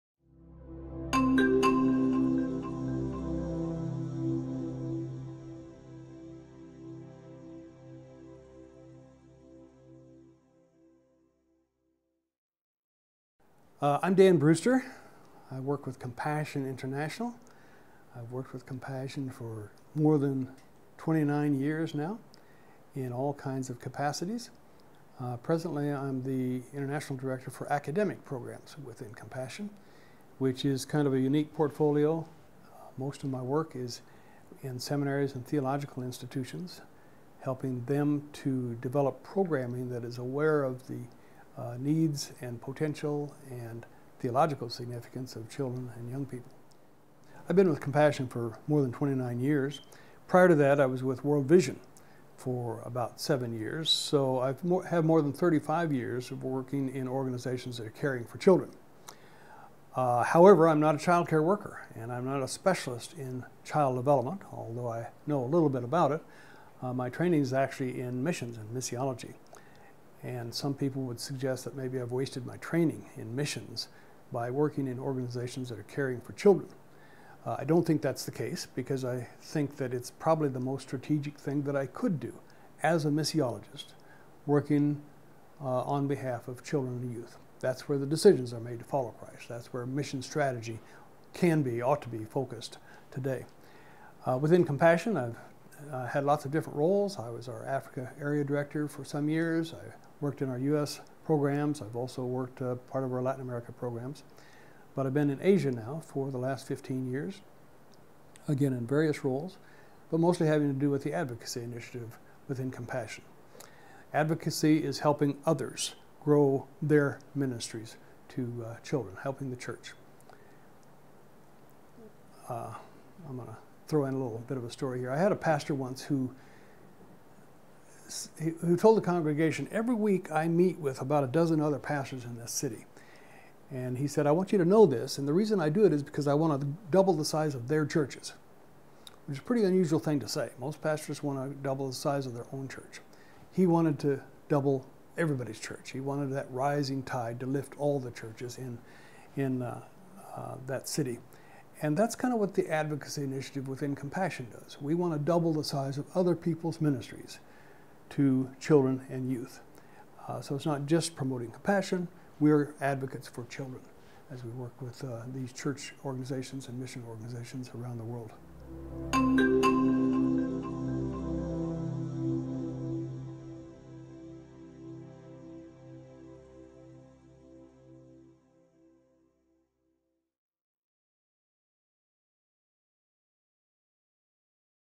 The Big Picture of Children in Scripture, Part 1: Intro and Advocacy explainED by Dr. Dan Brewster
In this series, Dr. Dan Brewster offers his expertise and insights on the topics surrounding children in Scripture. Here, in part one, he explains his role with Compassion International.